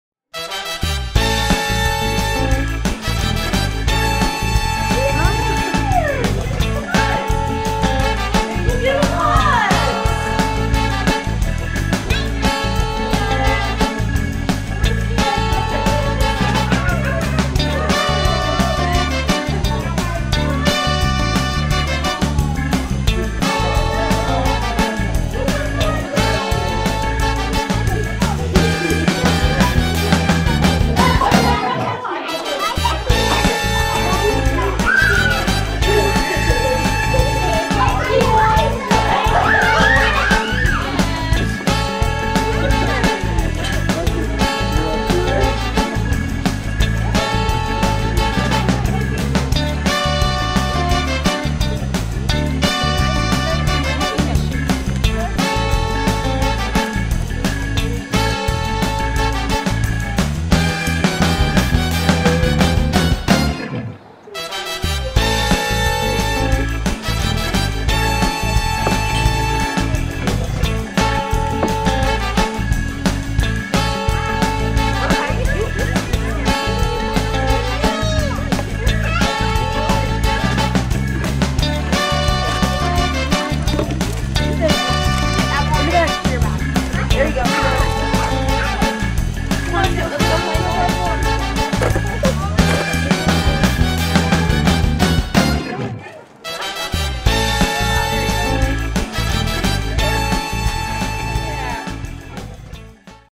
Holy Child Easter Egg Hunt
The Easter bunny makes a special visit at Holy Child Early Learning Center's annual Easter egg hunt.